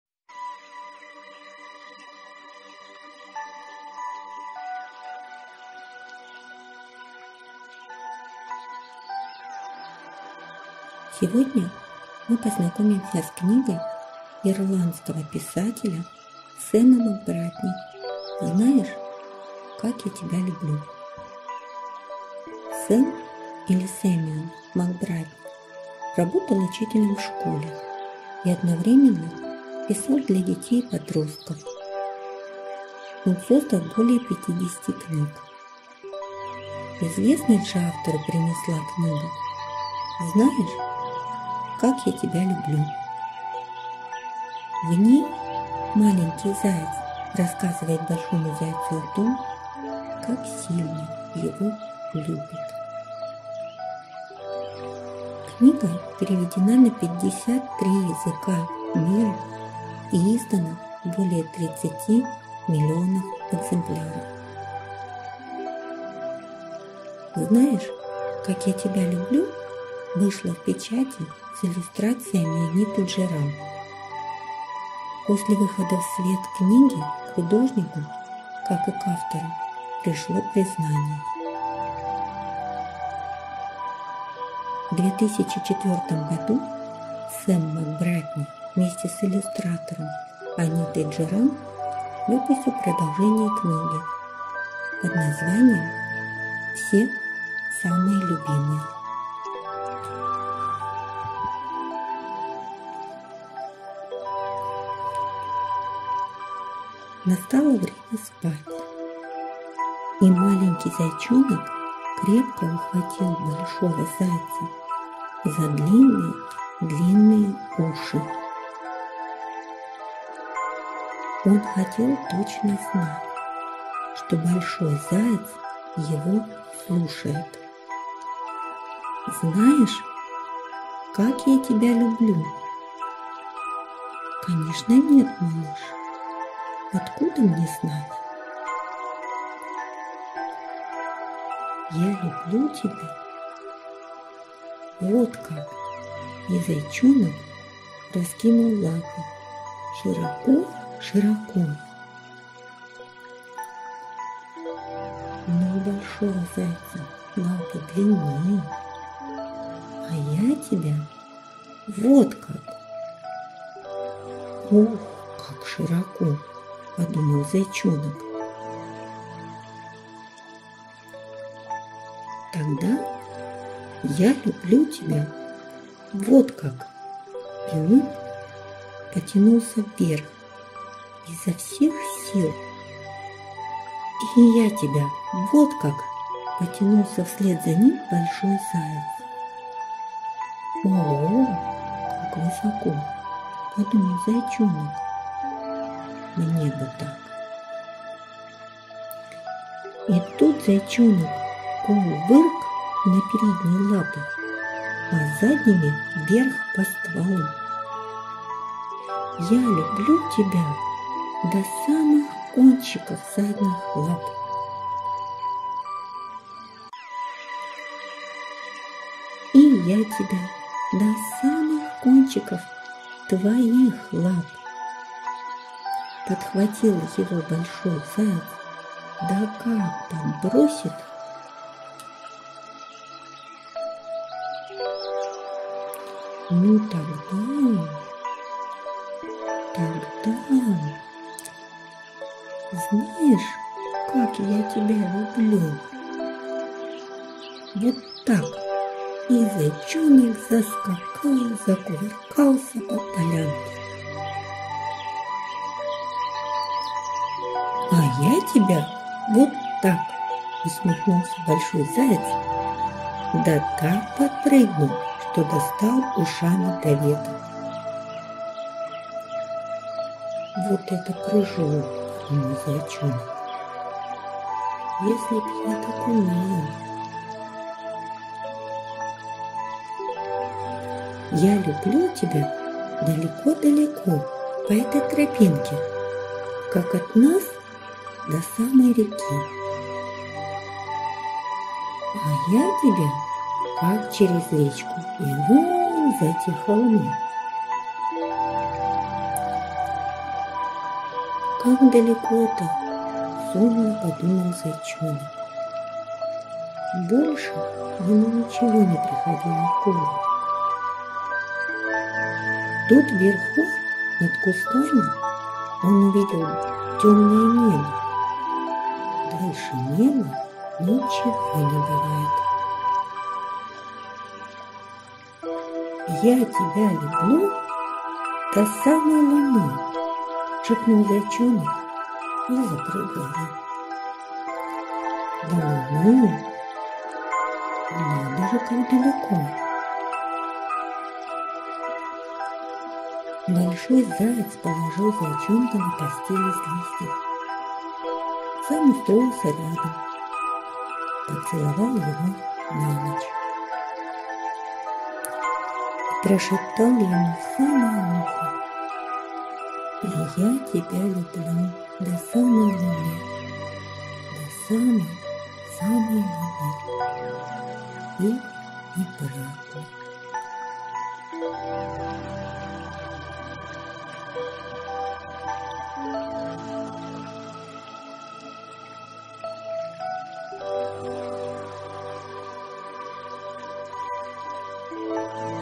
«Знаешь, как я тебя люблю?»
В Привокзальной детской библиотеке № 8 юным читателям предлагают познакомиться с книгой ирландского писателя Сэма Макбратни.